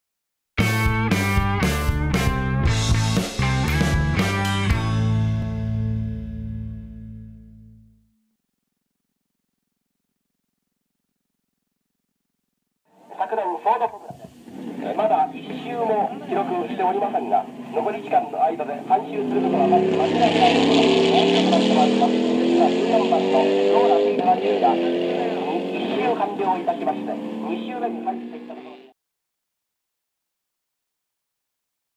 【ベストカー】名車のサーキットサウンド第８回 ローラＴ70 Mk.-Ⅱ 1967年５月２日
No.1クルマ雑誌 『ベストカー』の公式Youtubeアカウントです。